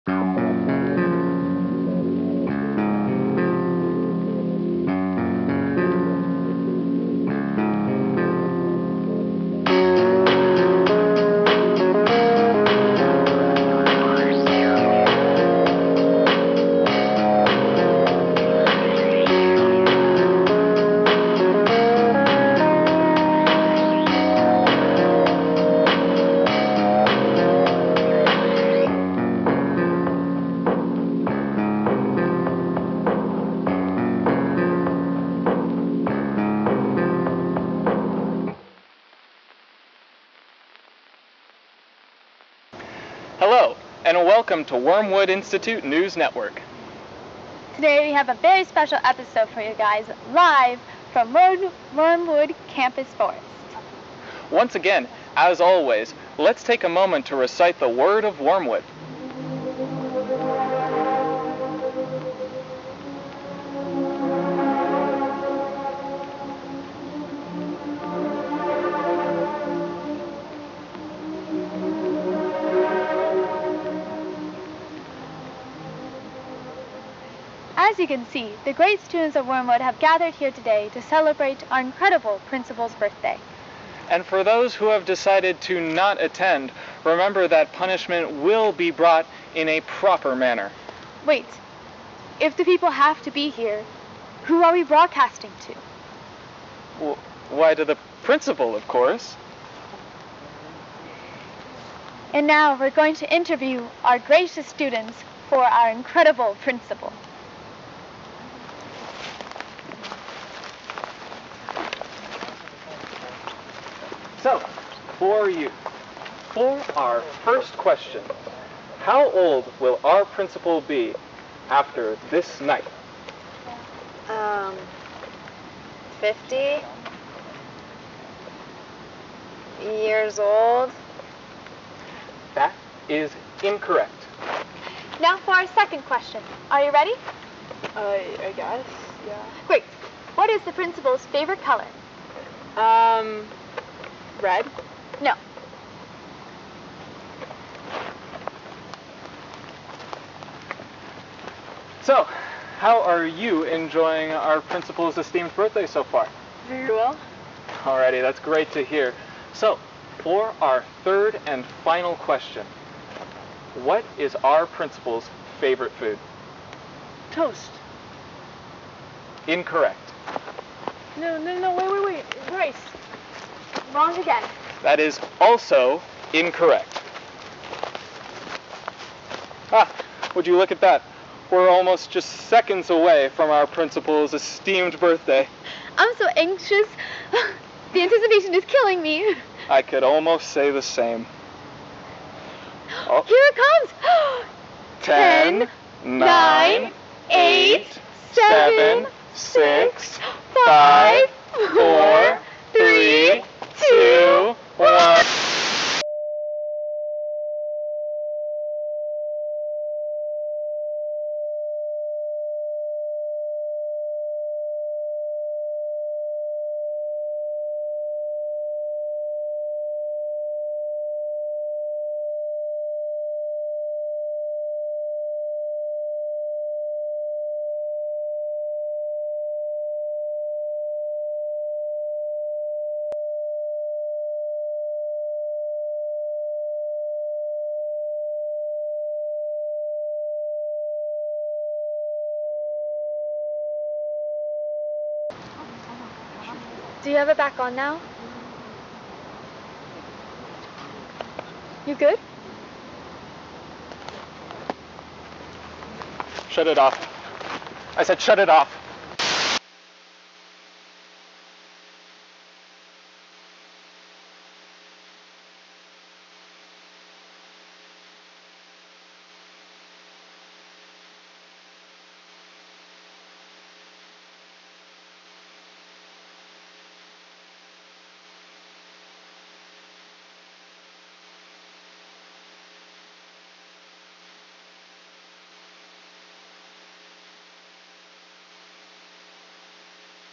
90’s Broadcast Gets Hijacked | Analog Horror
1/20/1994

A new analog horror series based on the infamous boarding school, Wormwood Institute, located in ███████████. From its now-abandoned campus, a handful of high school news broadcasts that aired in the 90s were recovered and digitized for archival purposes.

Wormwood Institute - Episode 3